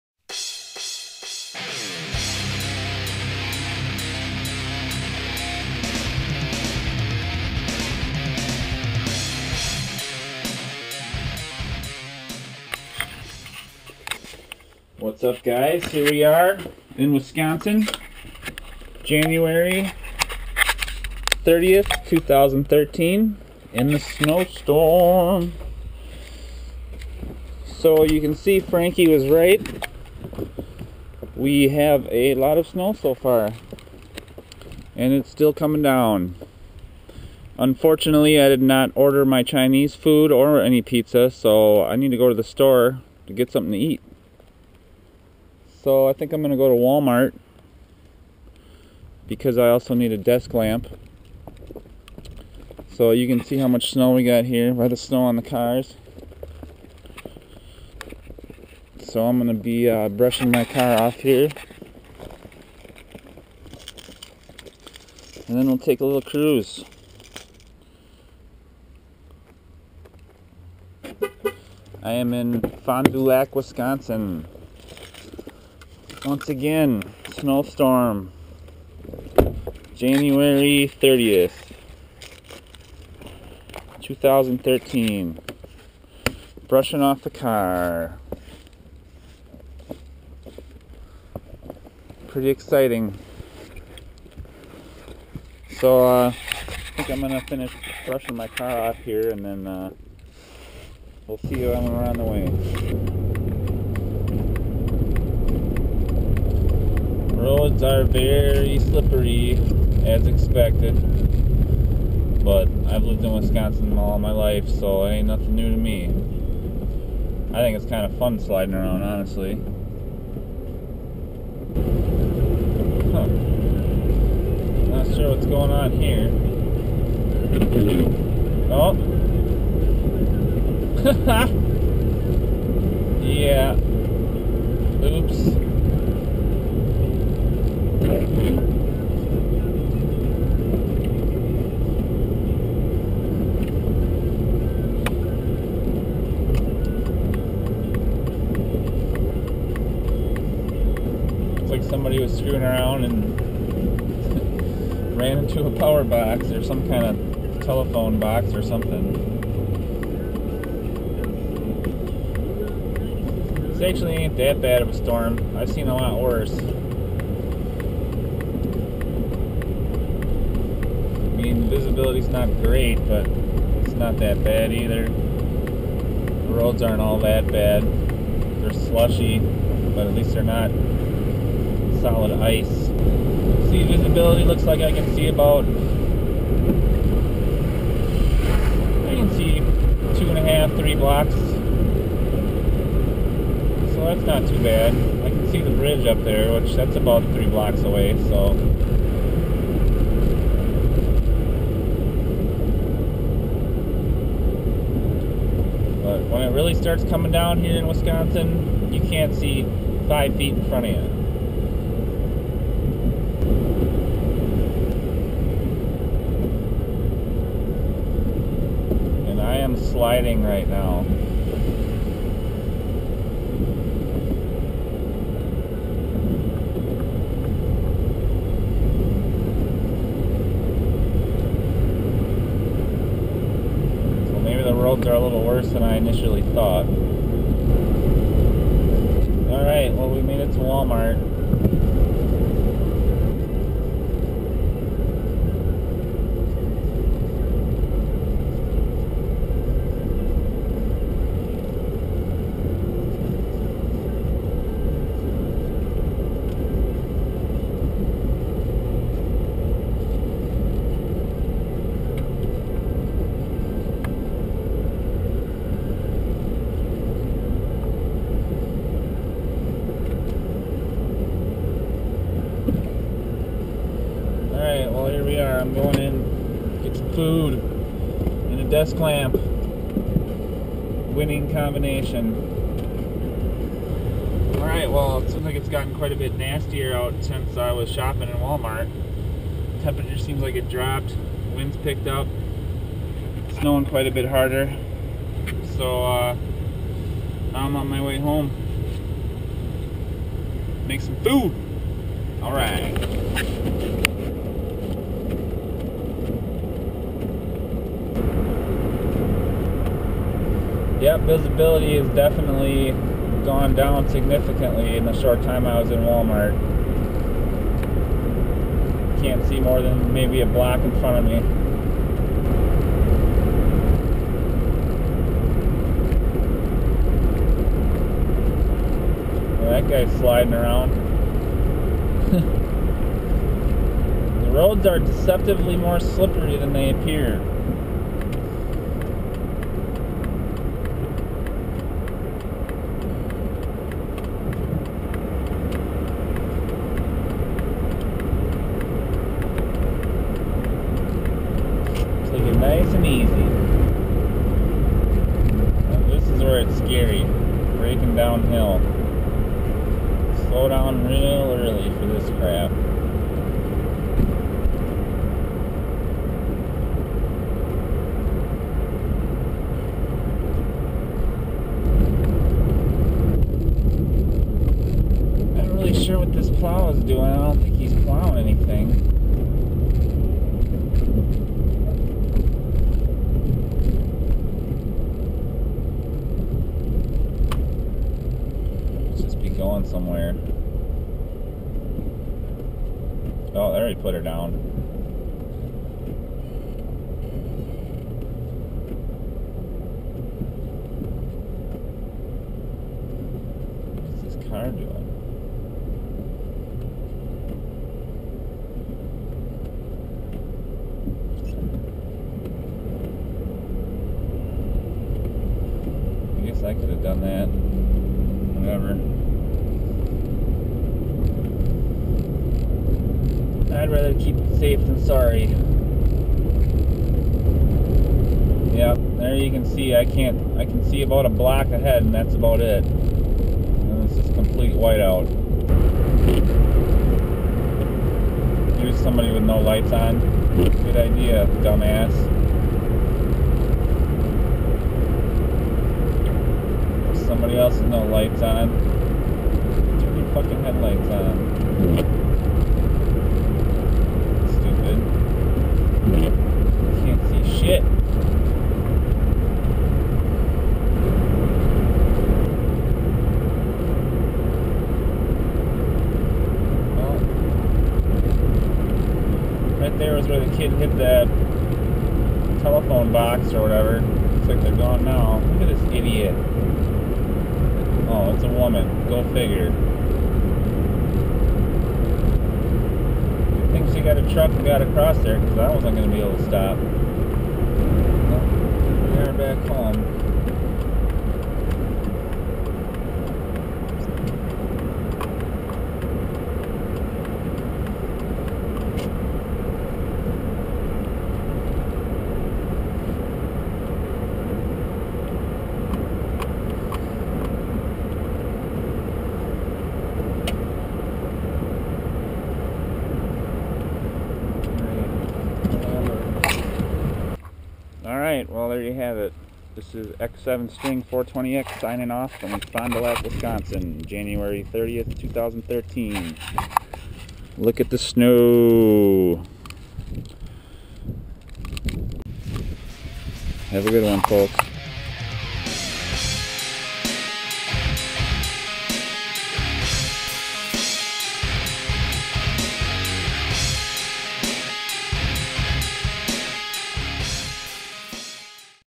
[HD] Driving in Wisconsin Snow Storm of 1/30/13
Going to Wal-Mart in the Wisconsin Snow Storm of 1/30/13.